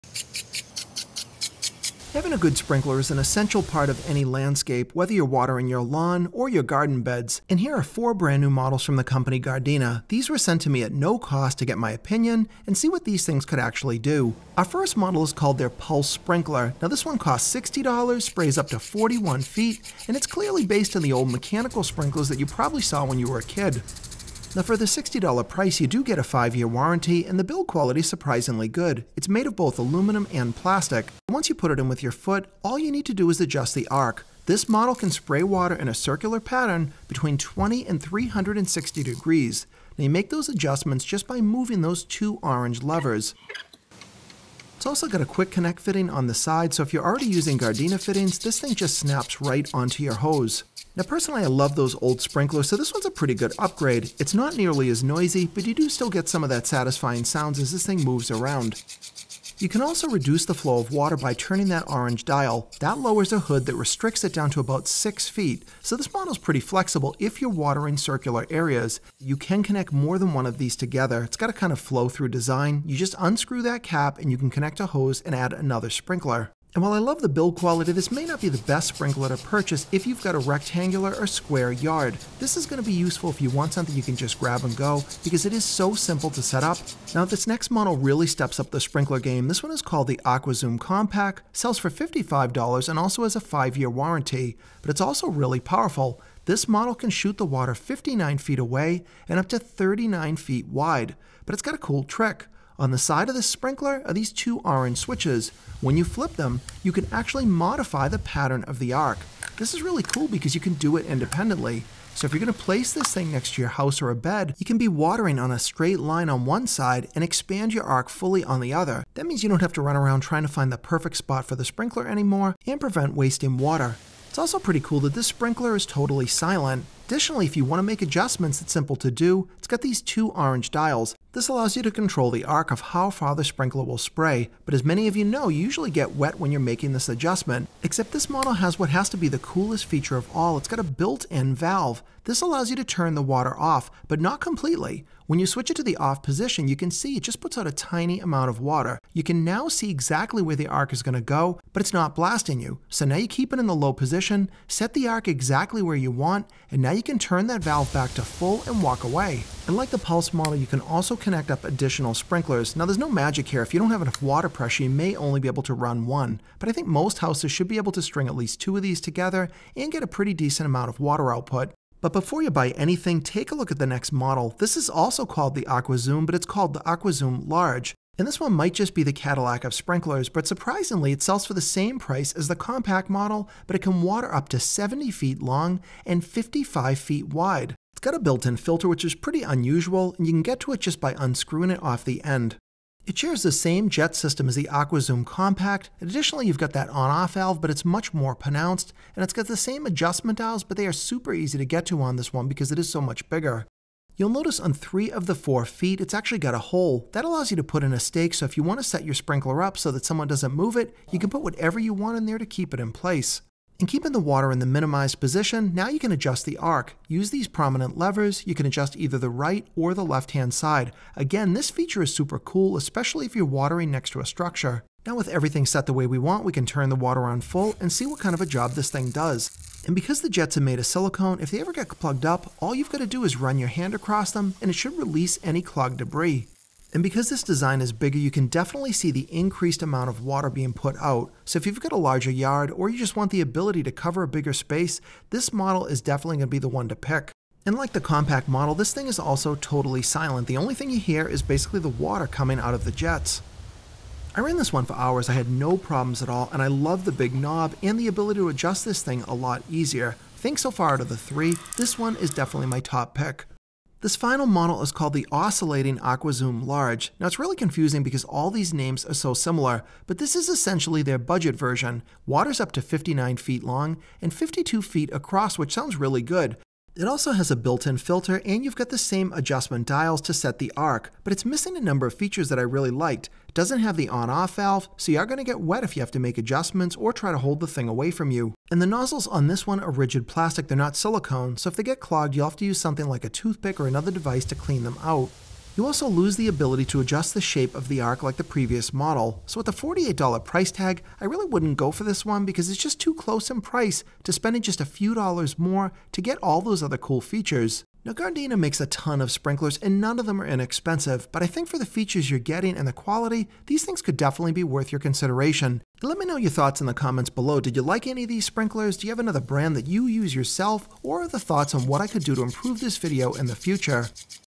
Trying Gardena new water saving lawn sprinklers
Gardena sent me their most advanced lawn sprinklers to check out. They have many smart features designed for ease of use & to save water, but are they any good? Let's find out.

Sprinklers:
Pulse Rotary Sprinkler (Special deal $34)
Aqua Zoom Compact
Aqua Zoom Oscillating Large
Oscillating Budget Sprinkler